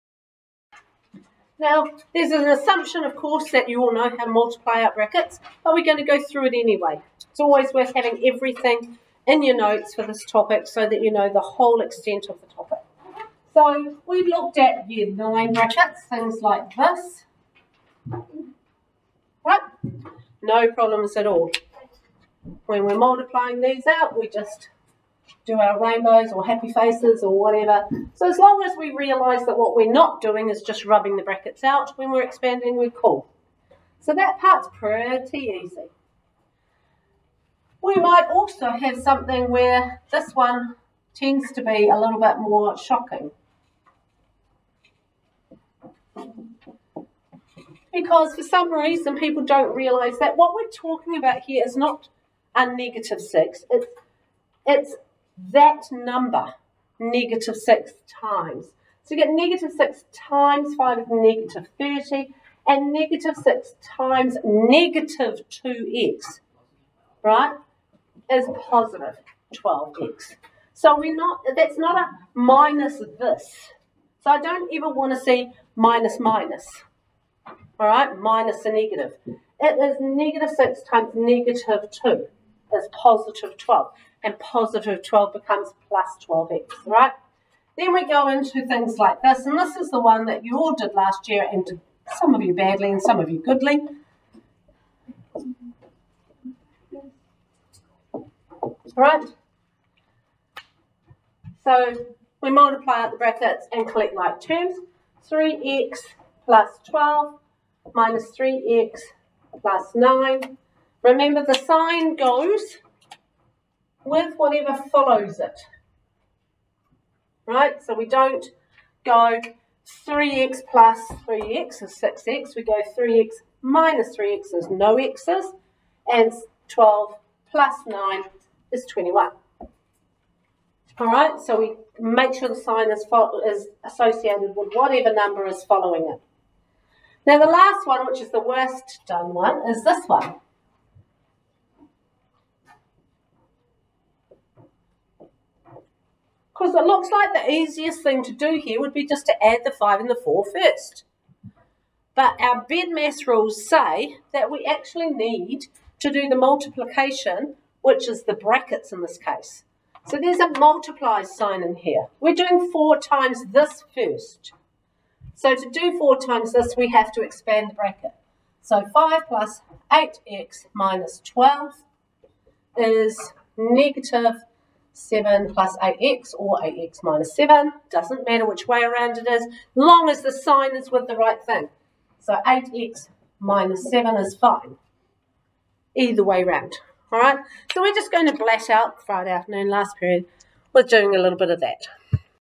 NCEA Level Two 2.6 Expanding Brackets
Starting from the beginning of Algebra and expanding linear and quadratics expressions by multiplying through brackets